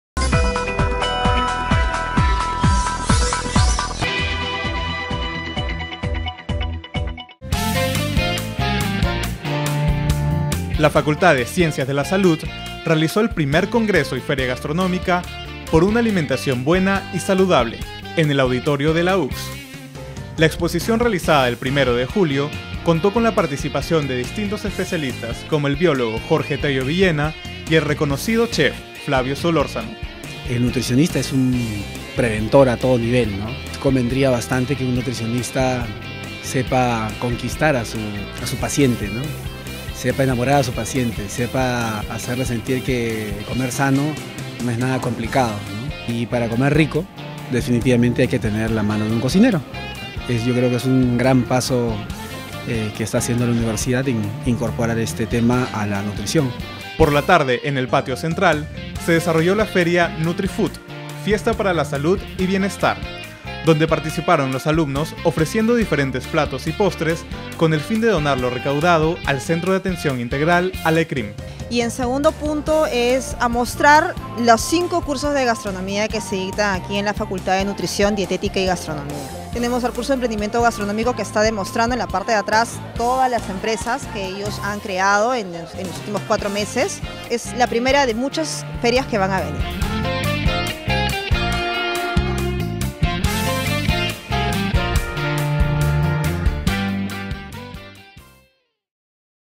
I Congreso y Feria NUTRIFOOD
Actividades para promover una alimentación creativa y saludable, a cargo de los alumnos de Nutrición, Dietética y Gastronomía.